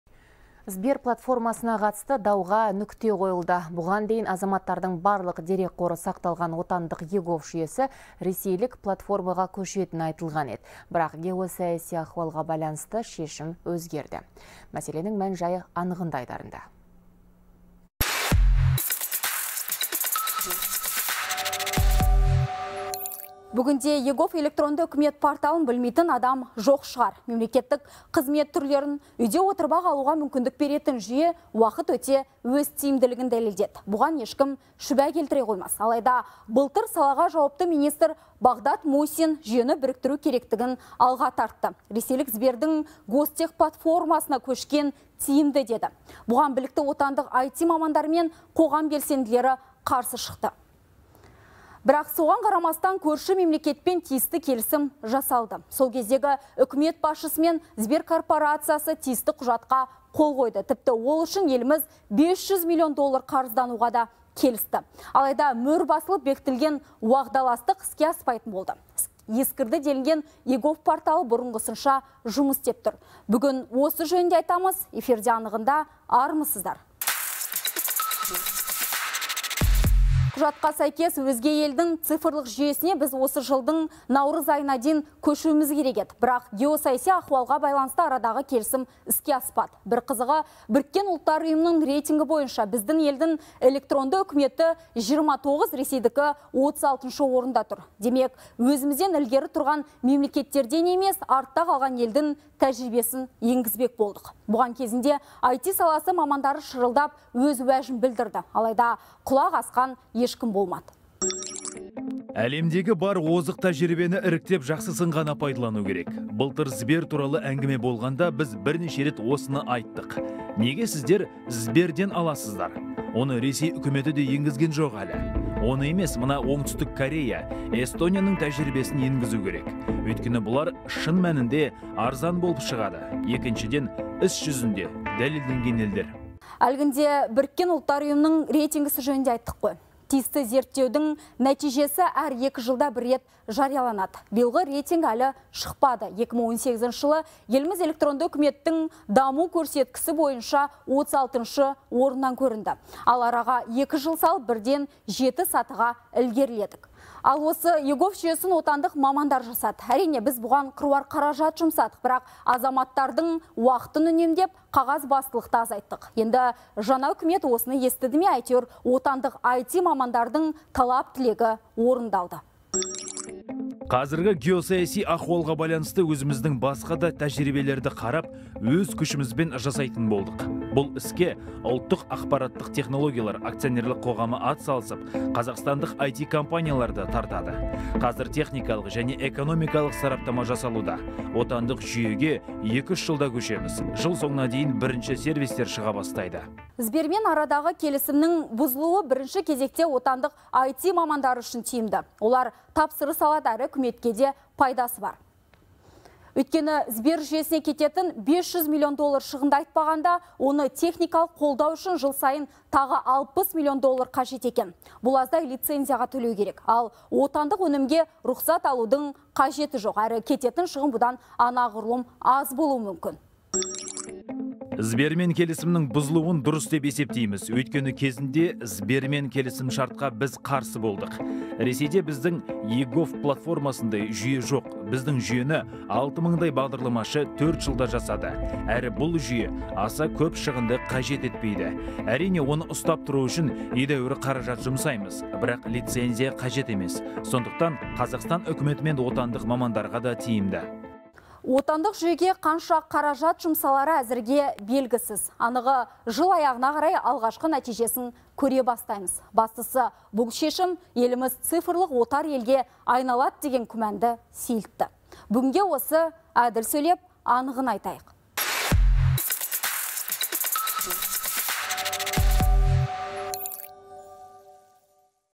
«СБЕР» жүйесіне нүкте қойылды
Бүгінде egov электронды үкімет порталын білмейтін адам жоқ шығар?! Мемлекеттік қызмет түрлерін үйде отырып-ақ алуға мүмкіндік беретін жүйе уақыт өте өз тиімділігін дәлелдеді. Бұған ешкім шүбә келтіре қоймас. Алайда былтыр  салаға жауапты министр Бағдат Мусин жүйені біріктіру керектігін алға тартты, деп хабарлайды «Хабар 24».